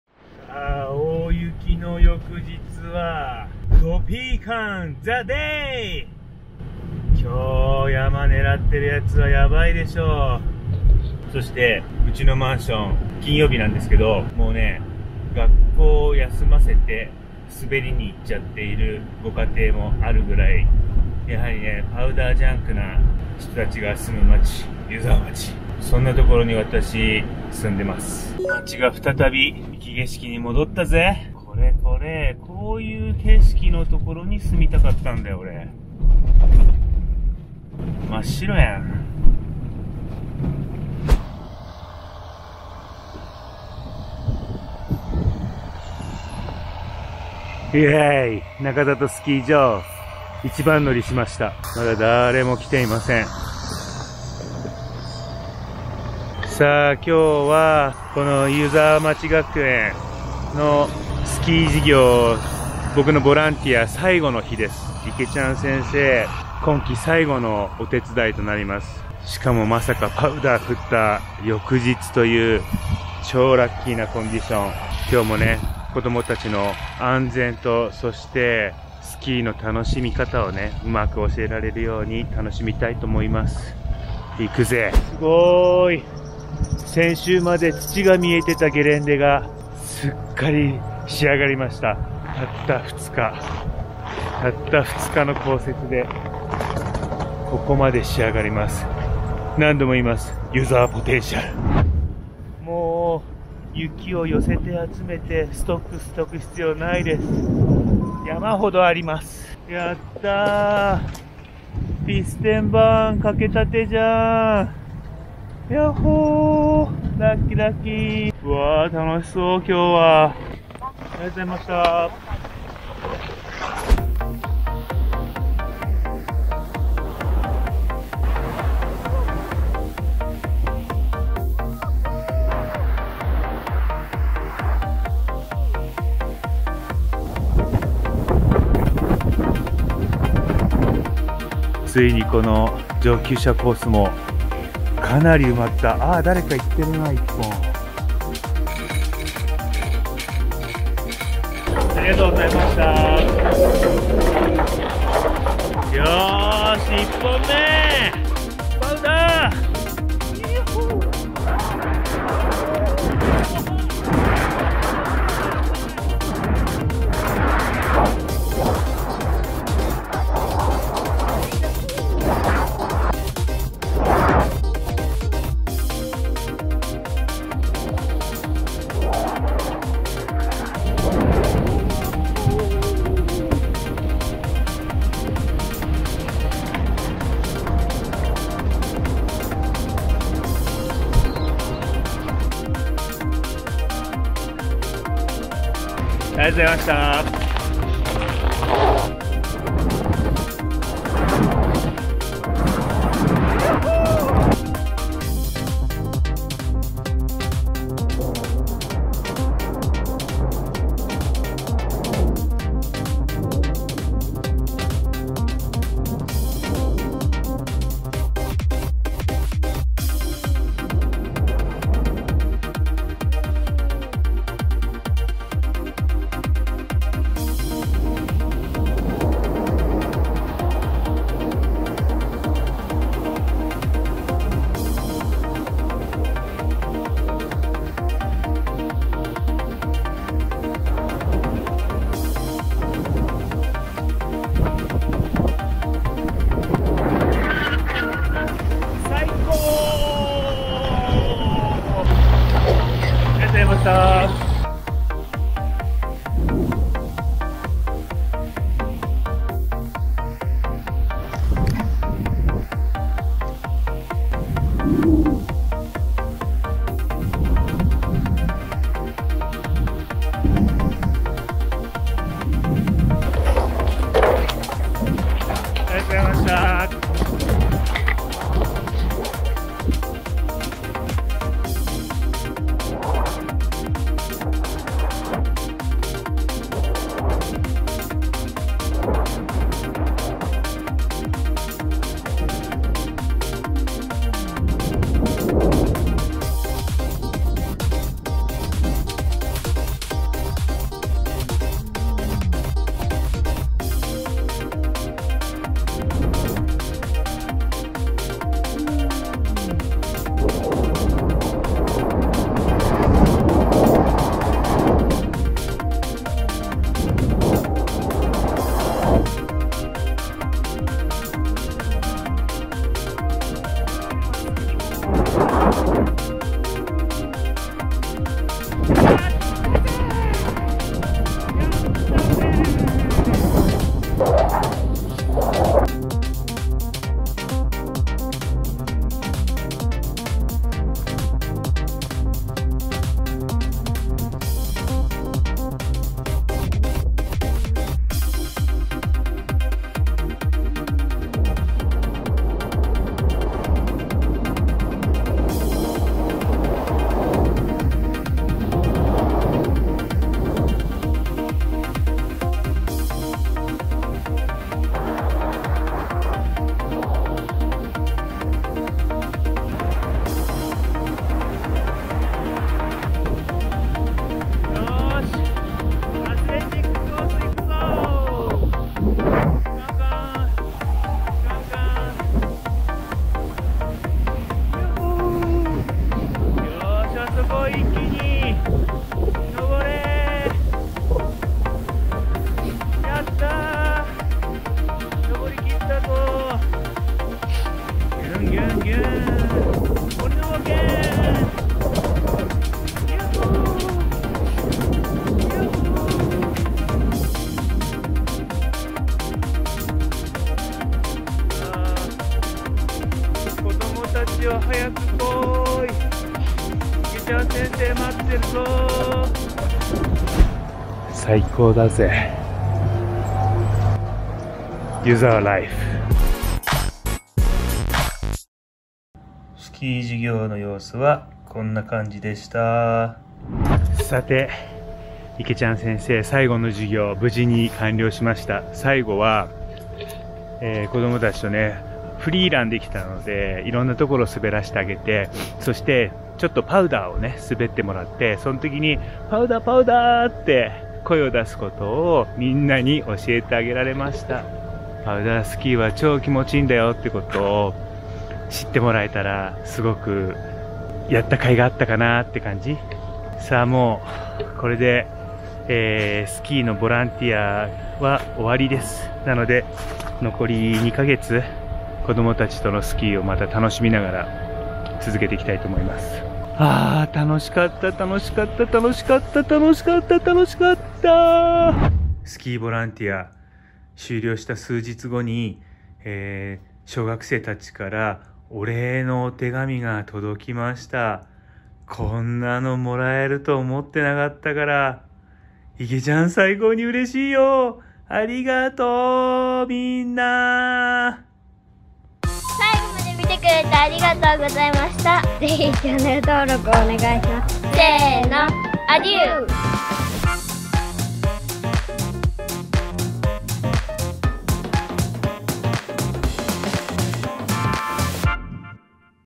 小学校最後のスキー授業もまさかのパウダースノー THE DAY!! #50
3年生最後のスキー授業もなんとパウダーでした！
授業の様子は撮影できなかったのでとうちゃんのパウダースキーをお楽しみくださいwww またお前のスキー動画かーい！って声が聴こえてきますw
動画の最後に生徒たちからのサプライズがあります、ぜひ見てください！

★日本おばかキッズ協会のブログ（とうちゃんがかいちょーをやってます）